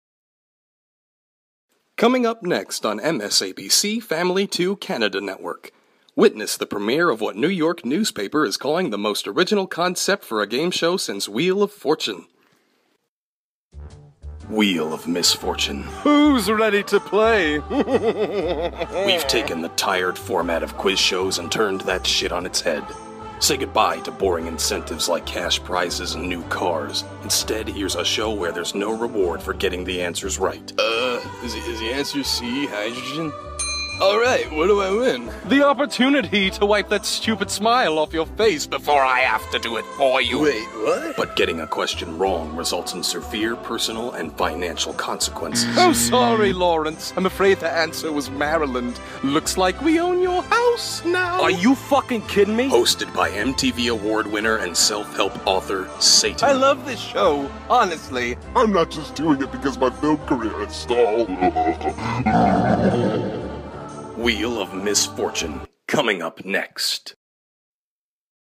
"Wheel of Misfortune" - Written and Voiced by J. Patrick Boyce
A sample for audio & voice over work/auditions.